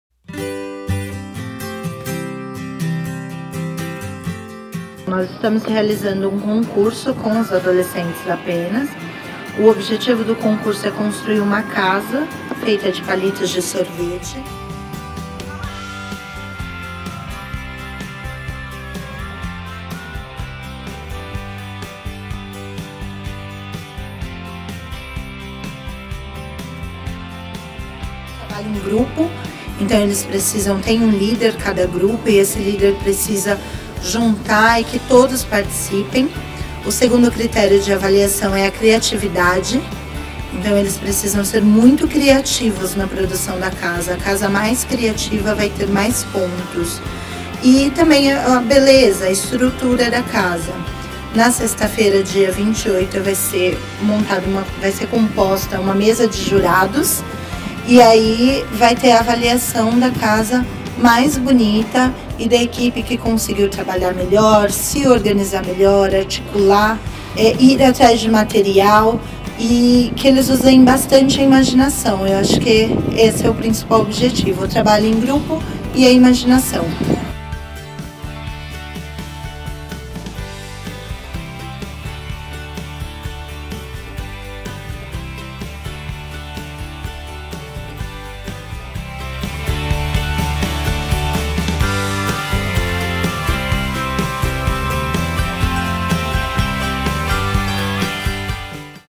Sefras Peri realiza concurso que busca exercitar a criatividade
Na última semana de agosto, o Sefras Peri realizou um concurso de artesanato com os adolescentes que freqüentam o serviço. O objetivo deste concurso foi exercitar a criatividade, a concentração e o companheirismo dos jovens cidadãos por meio da construção, em grupos, de casas feitas de palitos de sorvete.

“A idéia fazê-los usar a imaginação”, afirma a assistente social Yamin Alves. Ela conta que um dos principais critérios de avaliação é a criatividade. “Existe um líder em cada grupo e a tarefa dele é fazer com que todos participem da construção. Precisam ser muito criativos na produção, a casa mais criativa vai ganhar mais pontos”, explica.

O Concurso

A oficina foi encerrada no último dia 28 de agosto, com a realização do concurso. “Foi montada uma mesa com jurados e houve a avaliação da casa mais bonita e da equipe que conseguiu se articular melhor e com harmonia”, disse a assistente social, que garantiu que no decorrer deste segundo semestre haverá mais atividades como essa.